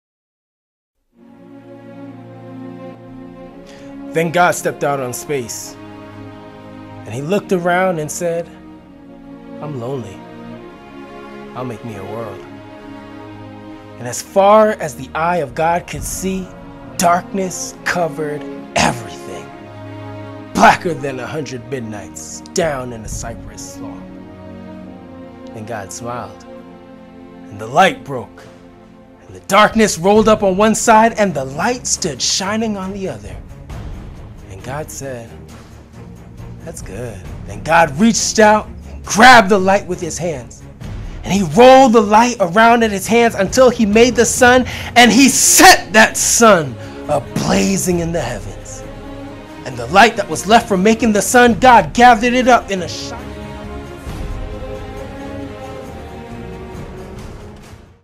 God's Trombone - "Creation"
God's Trombones
Seven Negro Sermons in Verse
By James Weldon Johnson
Saturday, February 18, 2017
Featuring great music and drama!

2015 Atlanta Berean Seventh-day Adventist Church | A Place for You